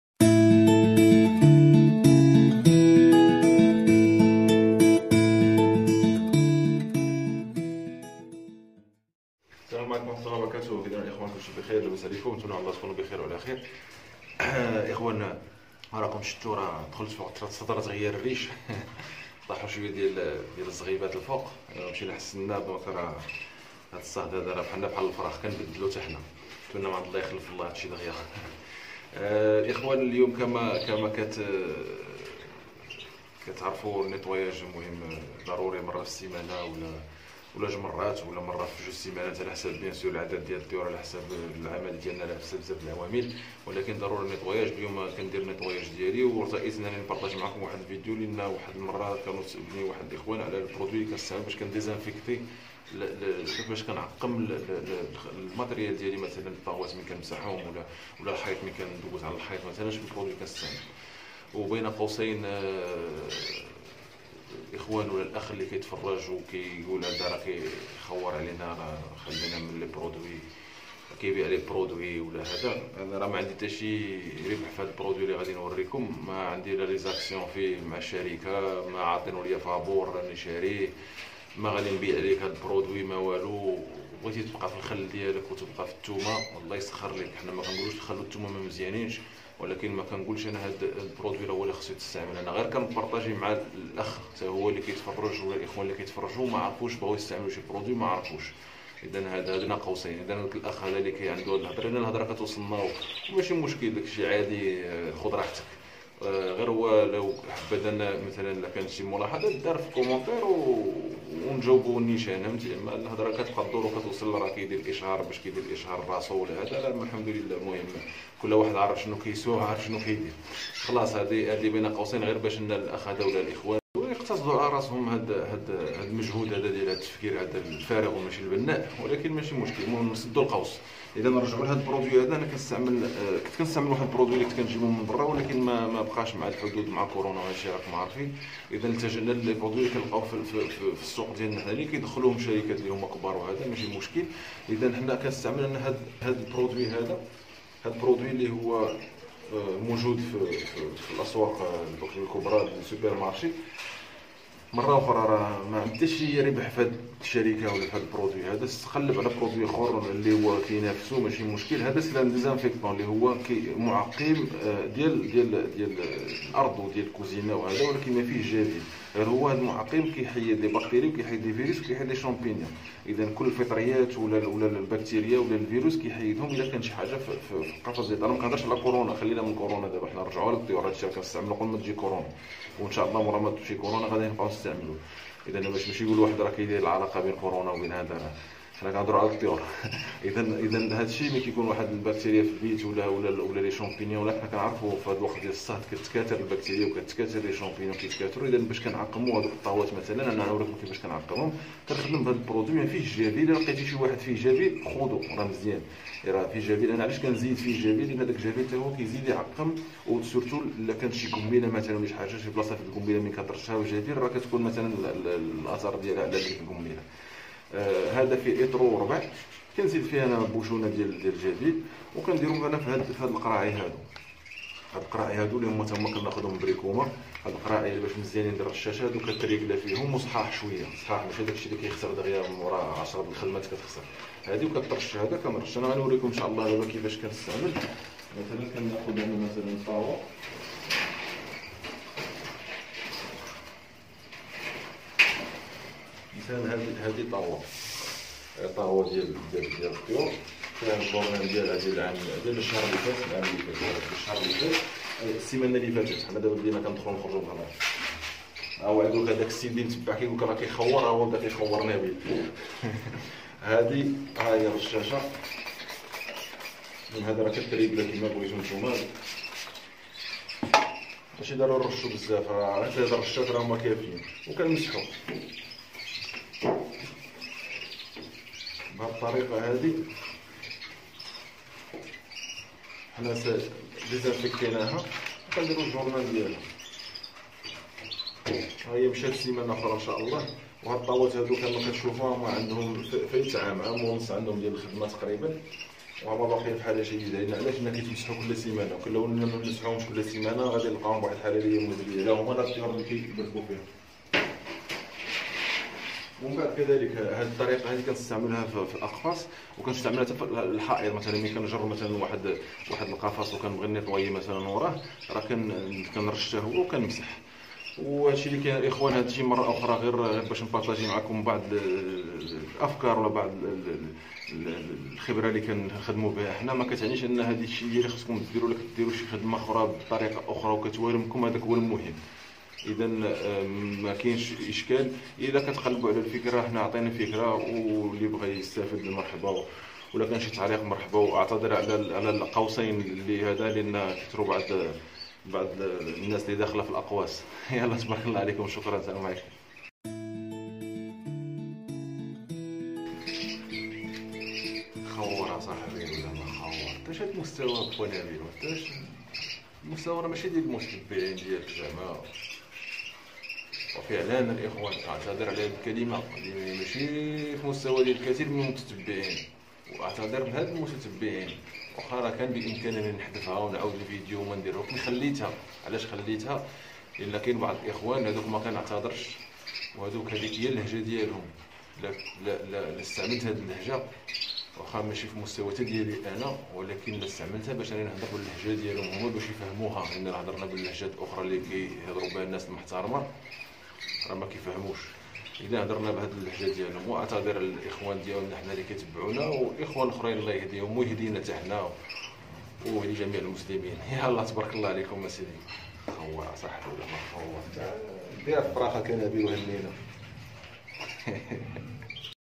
بمادا اعقم بيت الطيور
ملاحظة مهمة : من الاحسن ارتداء القفازات للعمل بالمنتجات المخصصة للنضافة. و إضافة جافيل ليس بالضروري لانه قد يكون ساما ادا كانت الجرعة كبيرة.
انا شخصيا لا اضيف الا القليل جدا و اضع دائما القفازات.